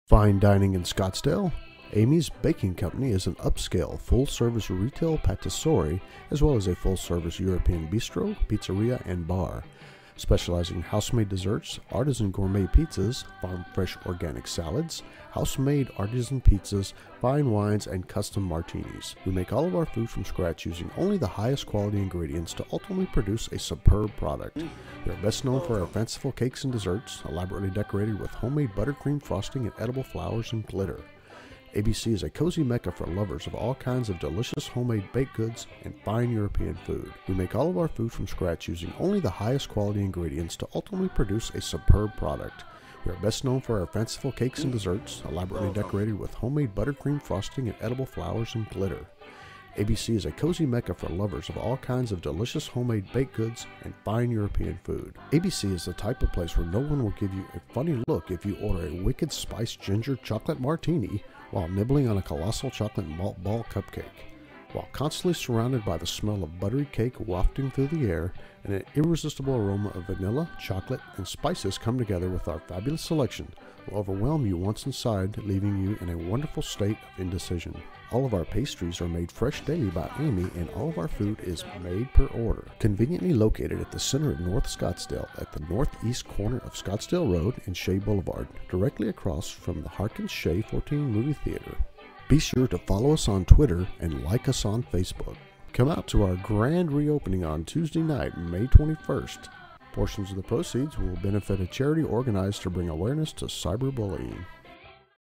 Amy's Baking Company - Spoof Grand Reopening Commercial
For the dialog, I read their  "about" page almost word for word.  Images were from their stolen images on the website and from their Facebook page.  We all know where the video came from.  This is all in fun.  It is not real so please comment accordingly.

Austin Rocks Entertainment is in business to make you laugh, sing, and dance.  This video may make you do two of the three,   Parody video only.

I saw one flaw in the transition but I have wasted way too much time already.  It is what it is.